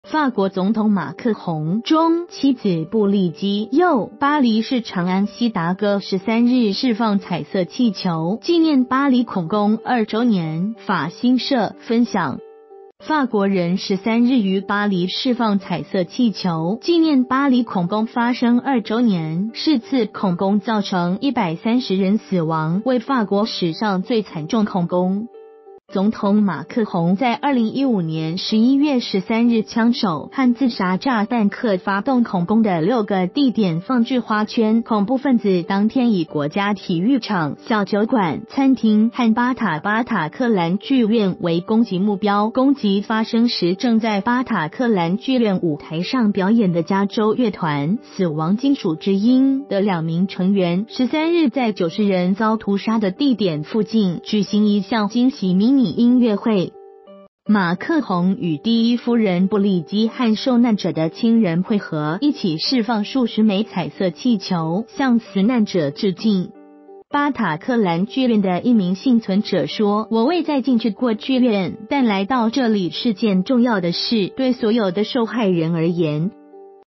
釋放彩色氣球 法國紀念巴黎恐攻2周年
法國總統馬克宏（中）、妻子布莉姬（右）、巴黎市長安．希達戈13日釋放彩色氣球，紀念巴黎恐攻2周年。（法新社） 分享

發表評論評論如下。